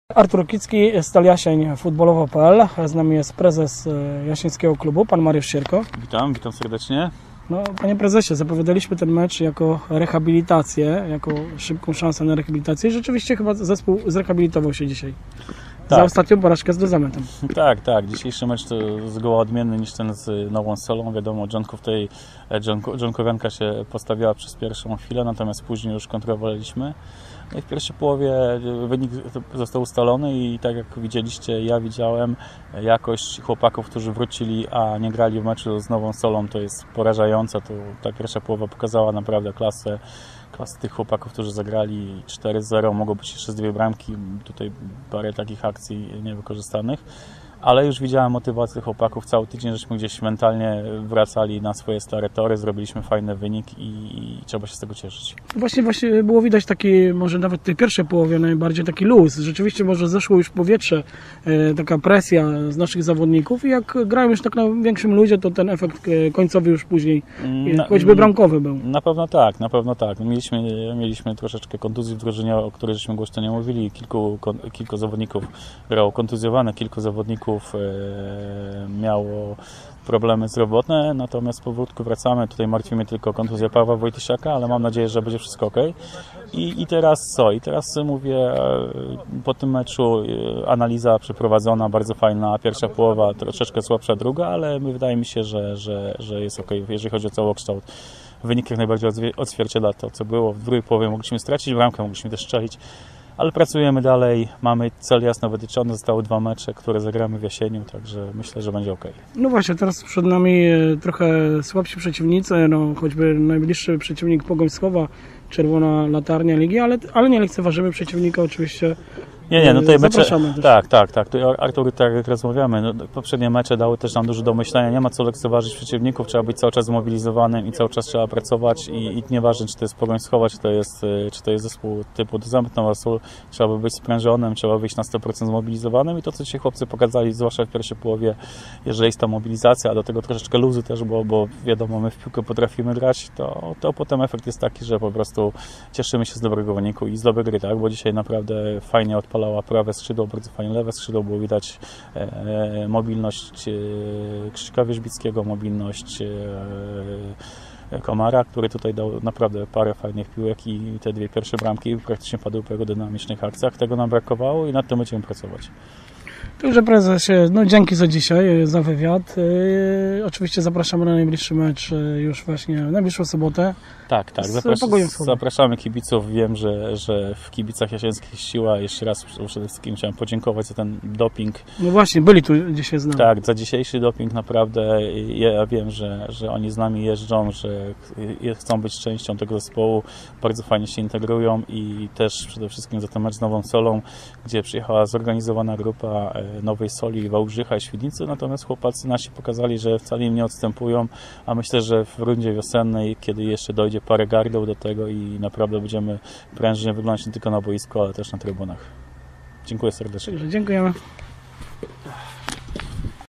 Rozmowa telefoniczna z prezesem Mariuszem Sierko po meczu Drzonkowianka 0-4 Stal (05.11.2022)
Prezentujemy wypowiedź prezesa Stali Jasień po meczu ligowym z Drzonkowianką Racula.